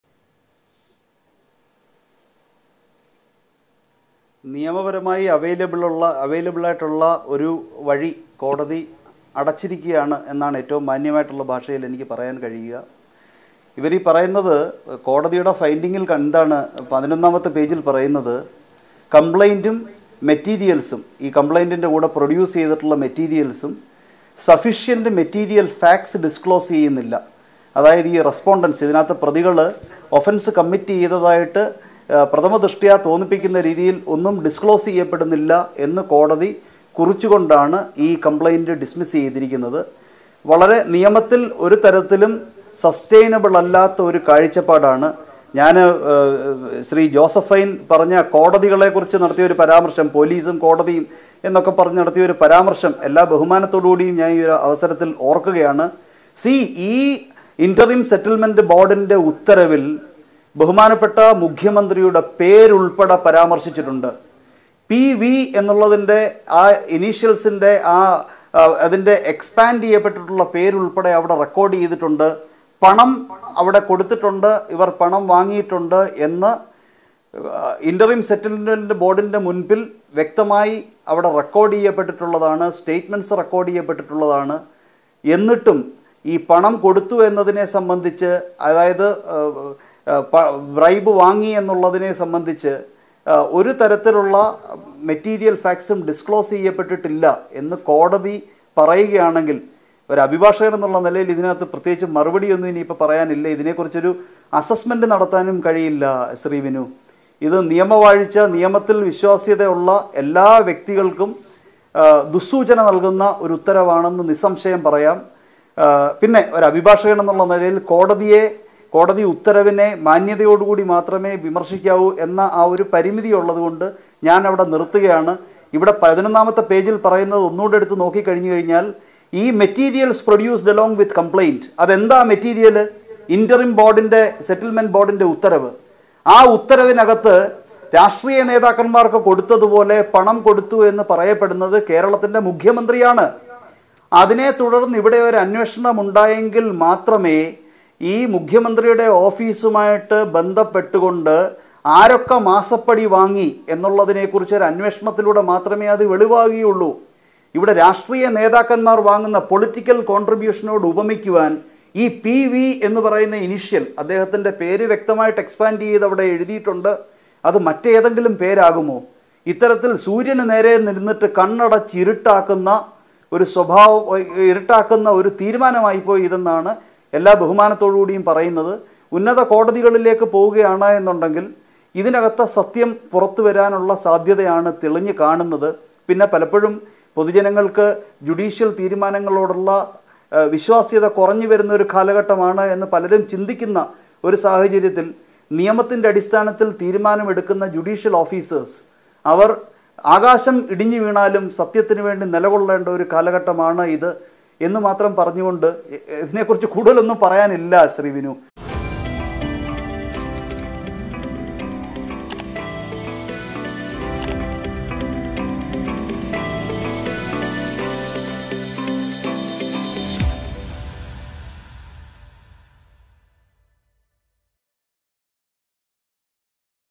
നിയമപരമായുള്ള ഒരു വഴി കോടതി അടച്ചെന്ന് എംആർ അഭിലാഷ് | MR Abhilash
'PV എന്ന പേര് പിണറായി വിജയനാണെന്ന് കൃത്യമായി രേഖകളിലുണ്ട്. എന്നിട്ടും തെളിവില്ലെന്ന് കോടതി പറഞ്ഞാൽ അഭിഭാഷകൻ എന്ന നിലയിൽ ഇനി ഇതിലൊന്നും പറയാനില്ല', നിയമപരമായുള്ള ഒരു വഴി കോടതി അടച്ചിരിക്കുകയാണെന്ന് അഡ്വ എംആർ അഭിലാഷ്